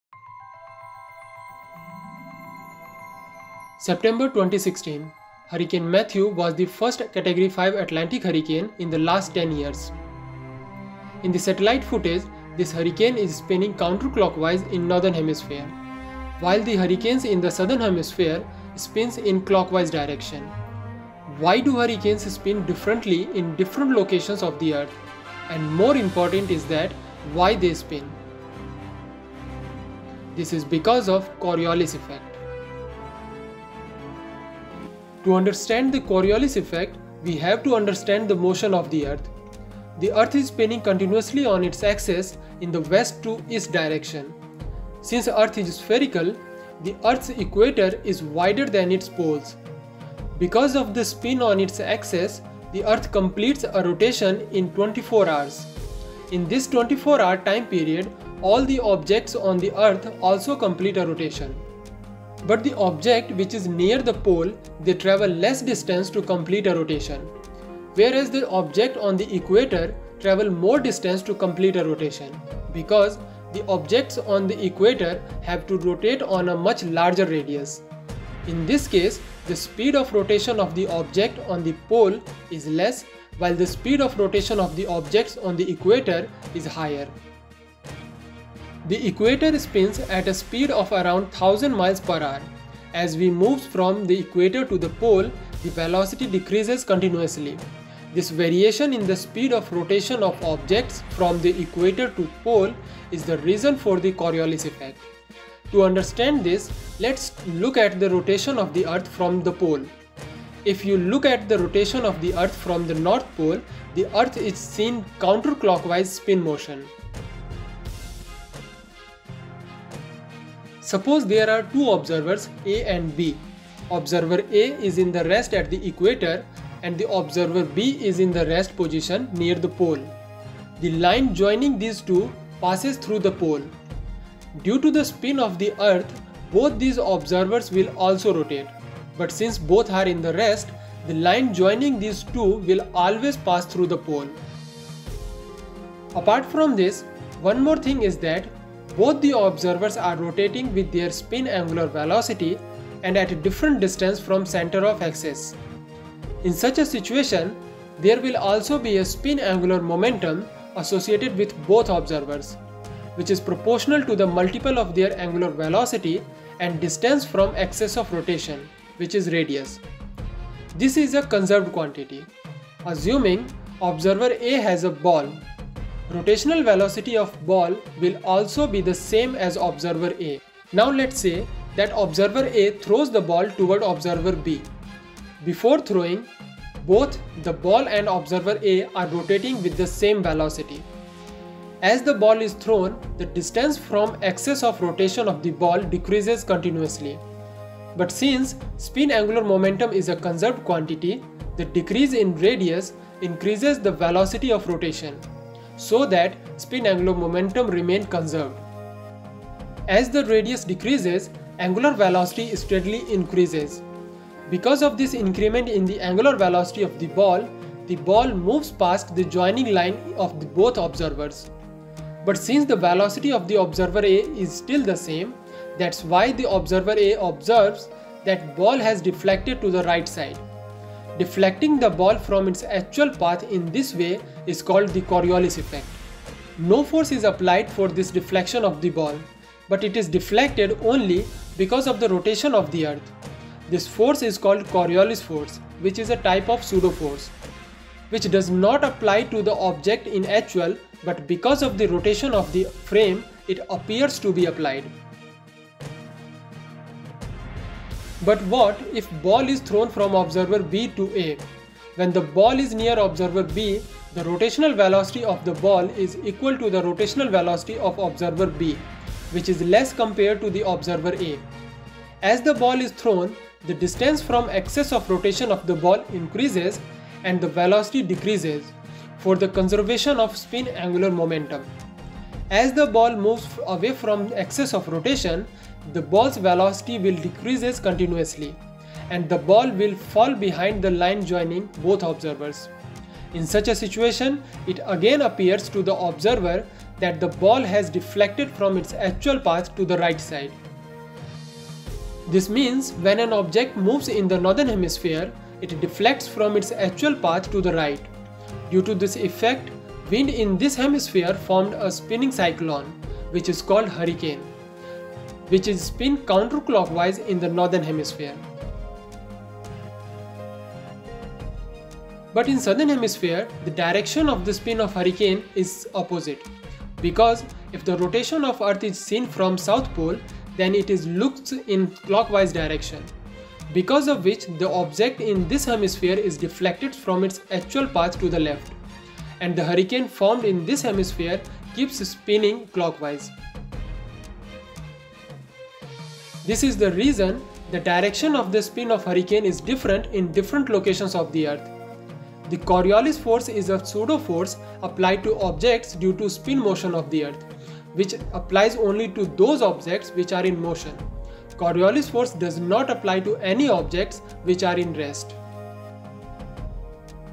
Coriolis Effect Explained
Earth rotation on its axis deflects moving objects from their actual path. This is called the Coriolis effect.